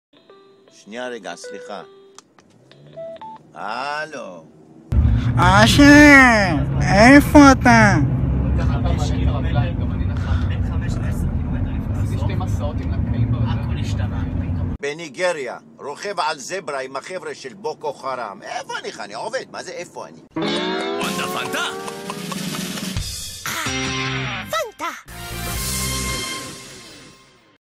אשר וחני
לכבוד היה לי לחקות את חני אשתו של אשר (ליאת הר לב המעולה) עם יובל סמו התותח